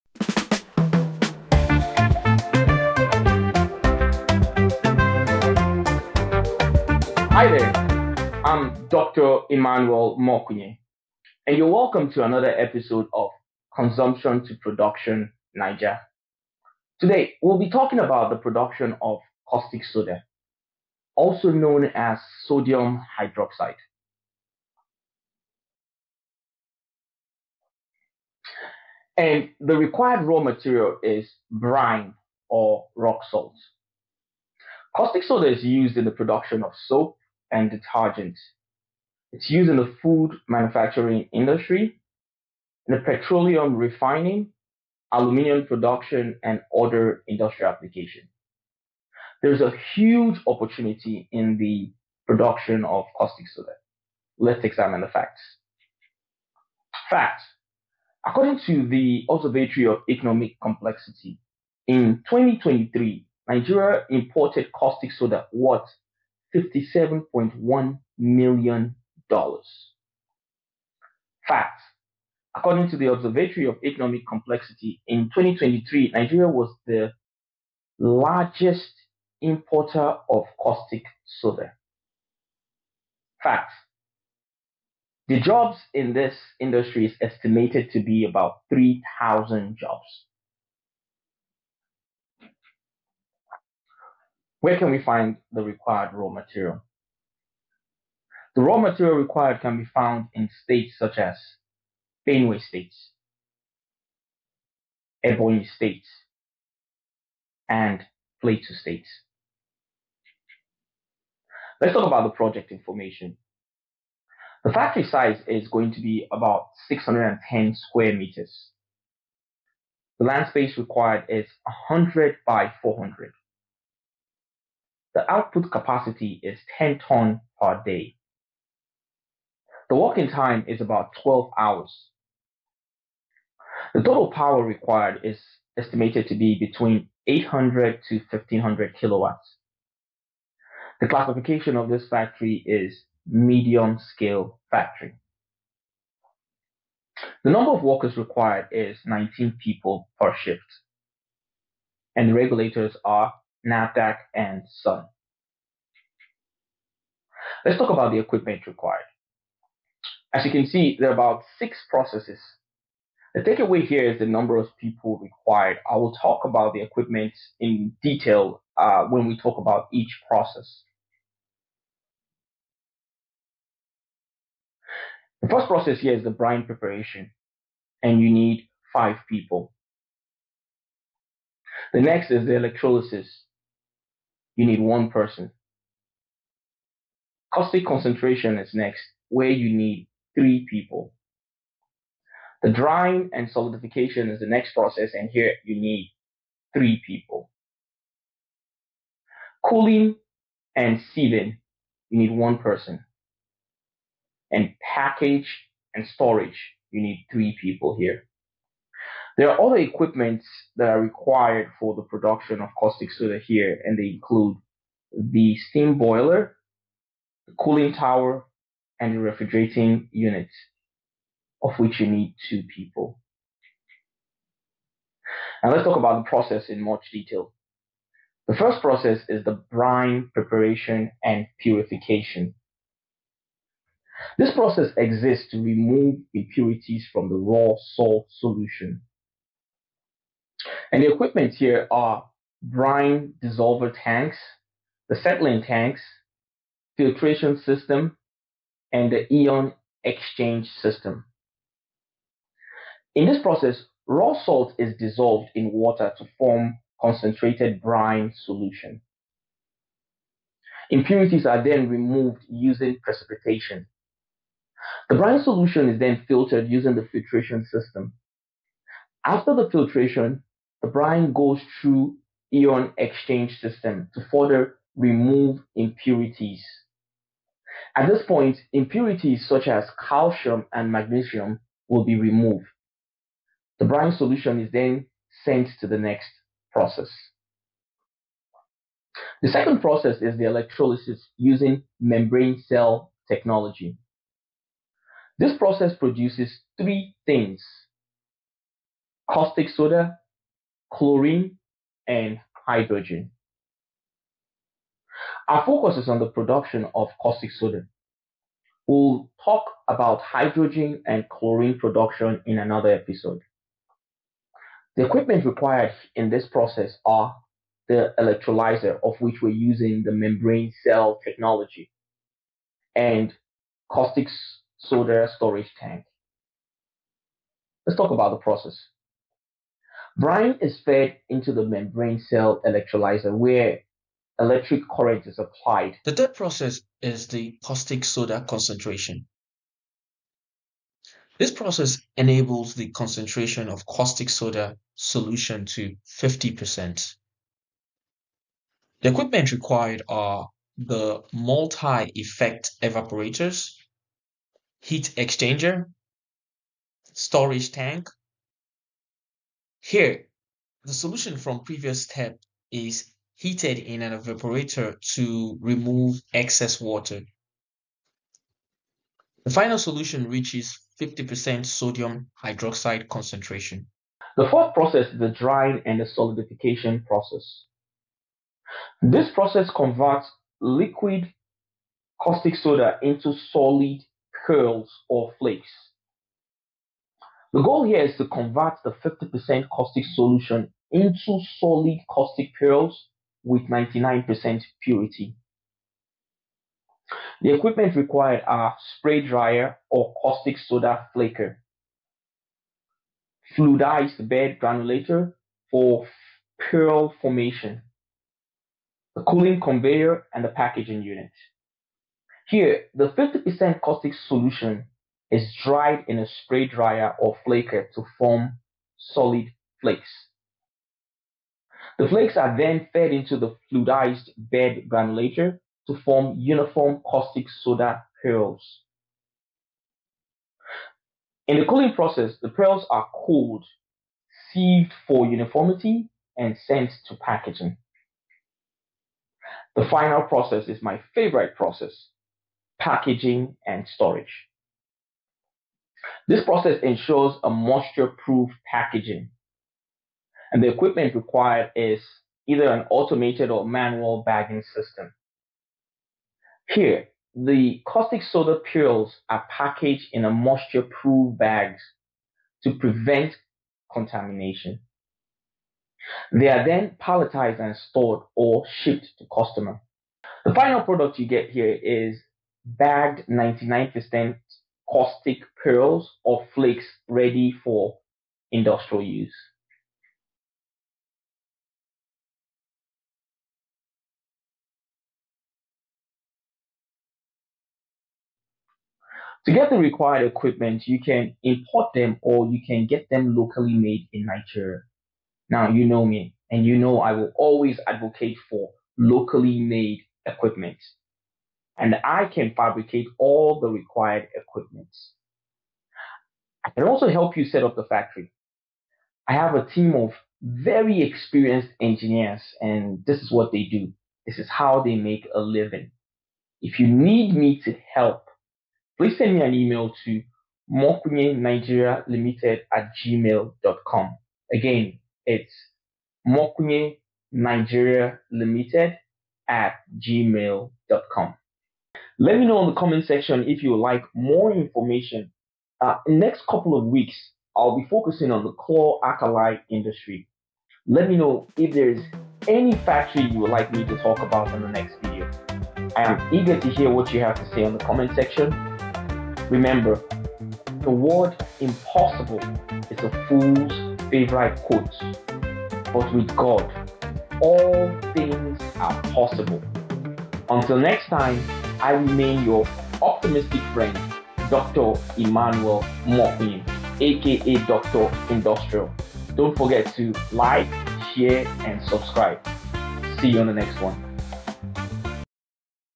Setting up Caustic Soda factory in Nigeria A Step By Step Guide
Welcome to Consumption to Production Nigeria!
In this video, we take you step by step through the process of setting up a Caustic Soda Factory in Nigeria. Whether you're an aspiring entrepreneur or an investor looking to tap into Nigeria’s growing industrial sector, this comprehensive guide covers everything you need to know—from sourcing raw materials and selecting the right equipment to facility layout, and regulatory compliance.

### Learn about:
✔ The fundamentals of **Caustic Soda production**
✔ Essential machinery and **setup requirements**
✔ Insights on market opportunities in Nigeria

Join me as I break down each stage of the setup process, providing practical tips and expert advice to help you build a sustainable and profitable caustic soda manufacturing business.